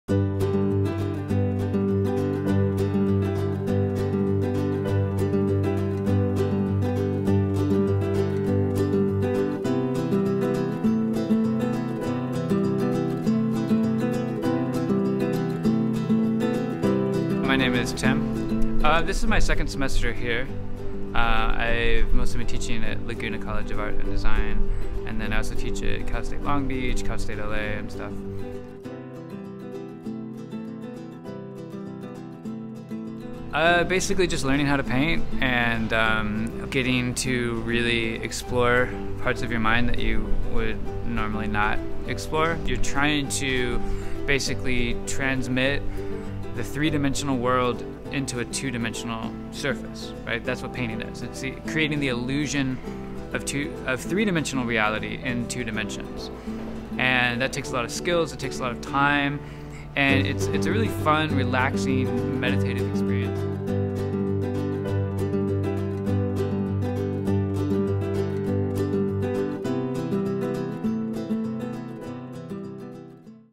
Saddleback College Fine Arts PROMO
Our team was given the opportunity to create a promo video for a department of our choosing within the Saddleback College Campus.

We interviewed Professor Timothy Robert Smith of the Fine Arts Department. Watch this short video to gain some insight on the painting class.